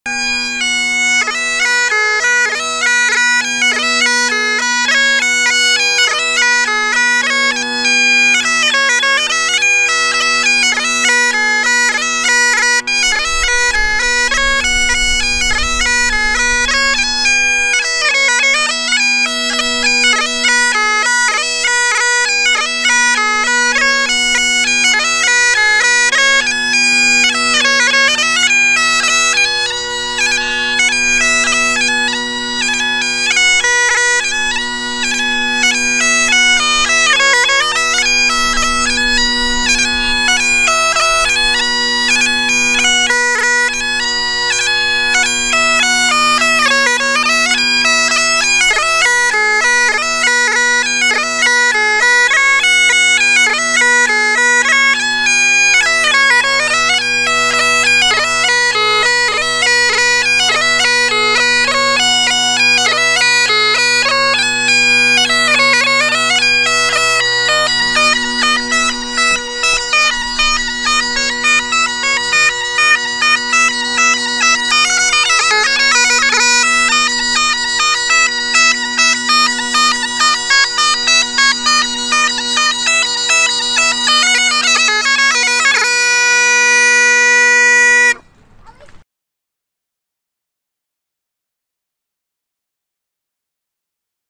Tradition: The Bagpipe, sounds of Glengarry Ontario Canada.
Bruce McCuaig a farmer from Lancaster, Ontario Canada playing the Bagpipes.

The Bagpipes are well known sound in Glengarry Ontario Canada.